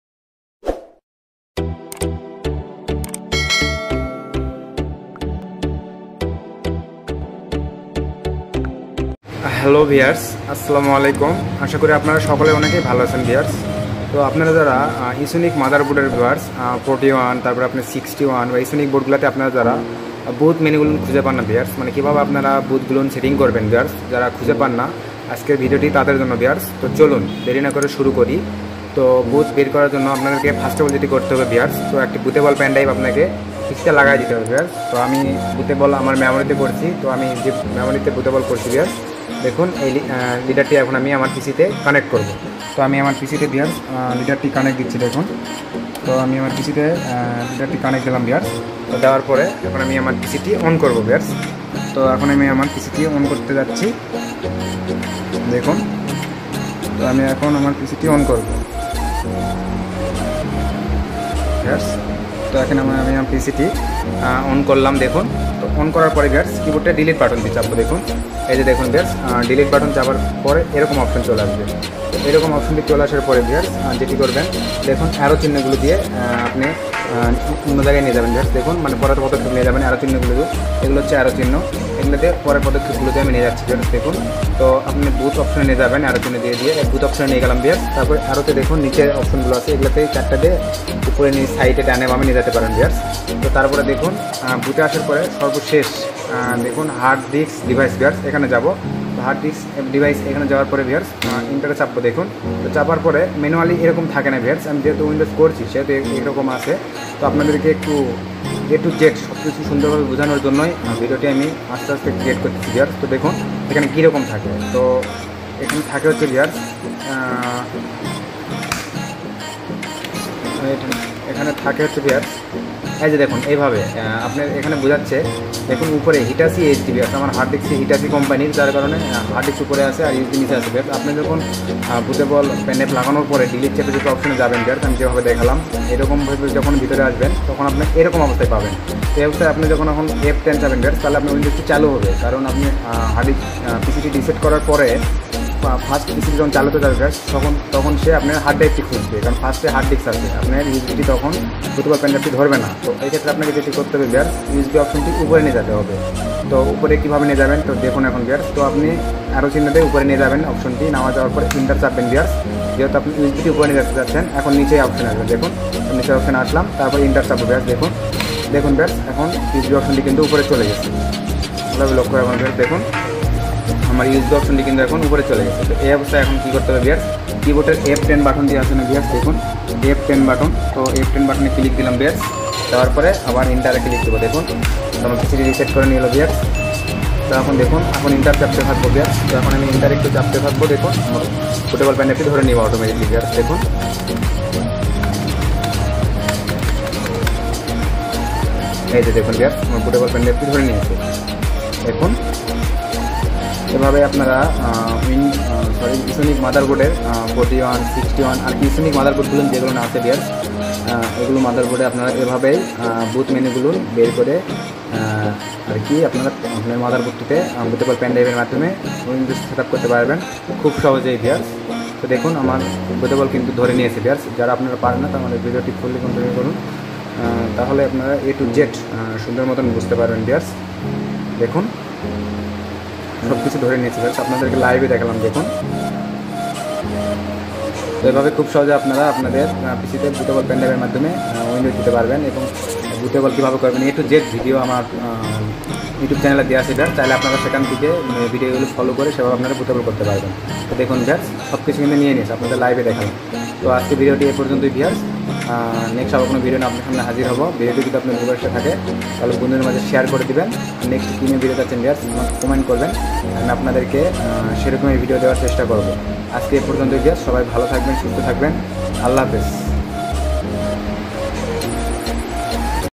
Esonic G41 & G61 Motherboard Boot and Bios Setup Easy way
How to Esonic G41and G61 motherboard boot and bios setup step by step..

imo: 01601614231

👉Ma it tech